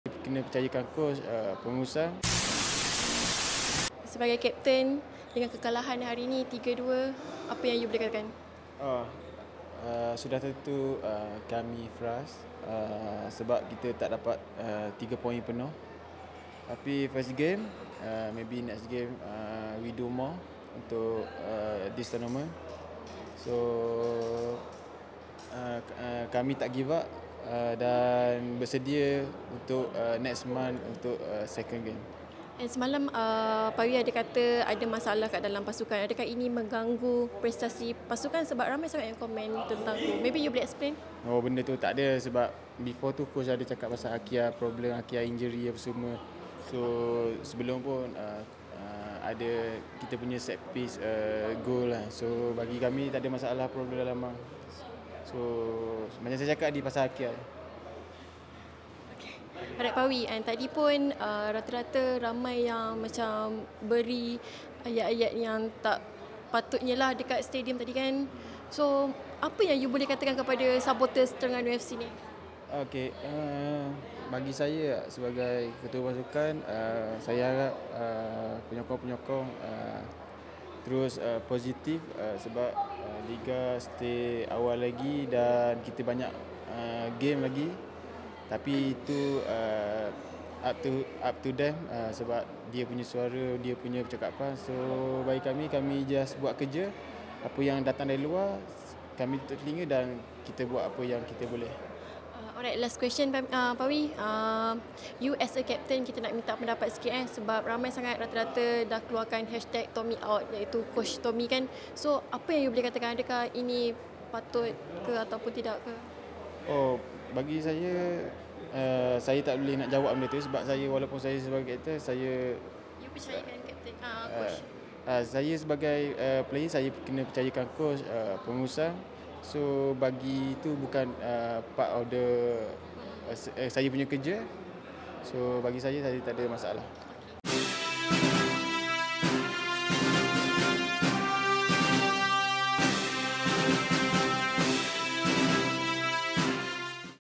"Bukan kerja saya untuk tentukan jurulatih kekal atau tidak! Harap penyokong terus positif!"
Temubual eksklusif bersama ketua pasukan Terengganu FC, Safawi Rasid selepas tamat perlawanan diantara Terengganu FC dan Svay Rieng FC bagi saingan ASEAN Club Championship (ACC) Shopee Cup 2024/2025.

Perlawanan berakhir dengan jaringan 3-2 berpihak kepada Svay Rieng FC.